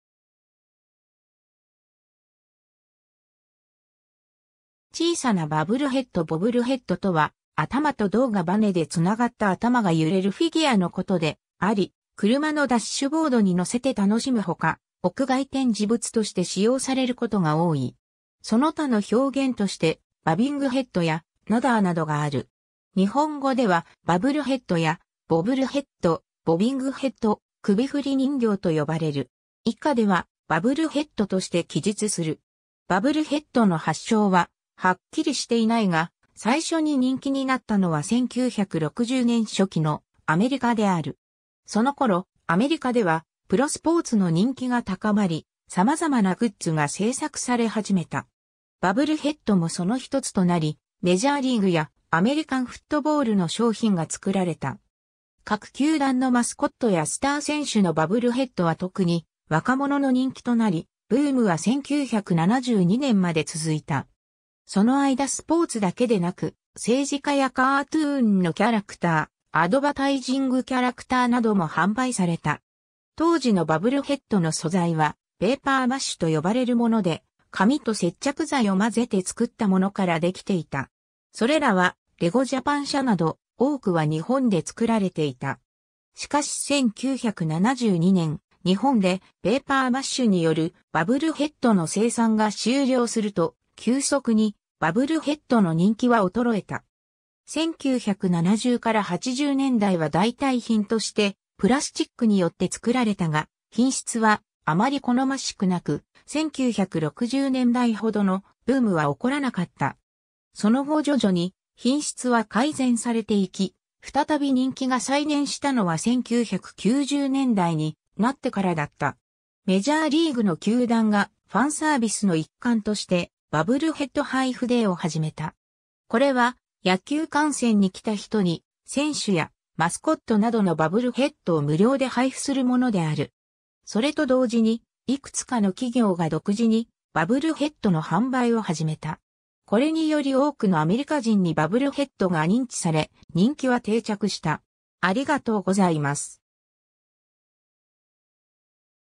Bobblehead
小さなバブルヘッド Bobblehead(ばぶるへっど)とは、頭と胴がバネでつながった頭が揺れるフィギュアのことであり、車のダッシュボードに載せて楽しむほか、屋外展示物として使用されることが多い。
 その他の表現としてbobbingheadやnodderなどがある。
日本語ではバブルヘッドやボブルヘッド、ボビングヘッド、首振り人形とよばれる。
以下ではバブルヘッドとして記述する。
 バブルヘッドの発祥ははっきりしていないが、最初に人気になったのは1960年初期のアメリカである。
そのころアメリカではプロスポーツの人気が高まり、様々なグッズが制作されはじめた。
バブルヘッドもそのひとつとなり、メジャーリーグやアメリカンフットボールの商品が作られた。
各球団のマスコットやスター選手のバブルヘッドは特に若者の人気となり、ブームは1972年まで続いた。
その間スポーツだけでなく、政治家やカートゥーンのキャラクター、アドバタイジングキャラクターなども販売された。
当時のバブルヘッドの素材はペーパーマッシュと呼ばれるもので、紙と接着剤を混ぜて作ったものからできていた。
それらはレゴジャパン社など、多くは日本で作られていた。
 しかし1972年、日本でペーパーマッシュによるバブルヘッドの生産が終了すると、急速にバブルヘッドの人気は衰えた。
1970～80年代は代替品としてプラスチックによって作られたが、品質はあまり好ましくなく、1960年代ほどのブームは起こらなかった。
その後徐々に品質は改善されていき、再び人気が再燃したのは1990年代になってからだった。
メジャーリーグの球団がファンサービスの一環として「バブルヘッド配布デー」を始めた。
これは野球観戦に来た人に選手やマスコットなどのバブルヘッドを無料で配布するものである。
それと同時にいくつかの企業が独自にバブルヘッドの販売を始めた。
これにより多くのアメリカ人にバブルヘッドが認知され、人気は定着した。